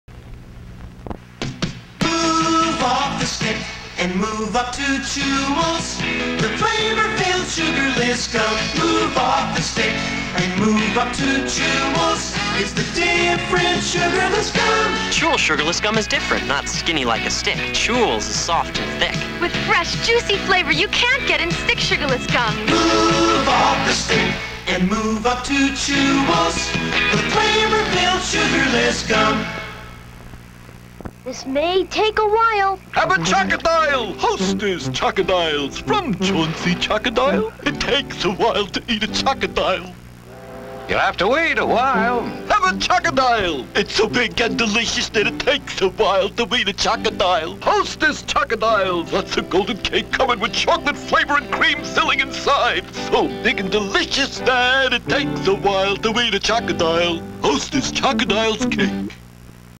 Vintage Chewels & Chocodiles Commercials (c.1970's)
Chewels is a defunct brand of chewing gum. It was the first gum that had a liquid center that oozed out when chewed. It was similar to the later gum called "Freshen Up".  A commercial for Chewels gum was one of the first advertisements aired on MTV. The commercial was later featured on MTV's website for their 25th anniversary, along with the first hour of programming when the network debuted.

Chocodiles /ˈtʃɒkɵdaɪlz/ are a confection created by the Hostess Brands company. The package describes the snack cake as a "chocolate coated sponge cake with creamy filling." The Chocodile is Twinkie shaped, but unlike the Twinkie which is sold in packages of 2 or 3 cakes, the Chocodile is sold individually. They can be unofficially called "chocolate covered Twinkies."

Two vintage commercials, spliced together for your enjoyment from 16mm prints.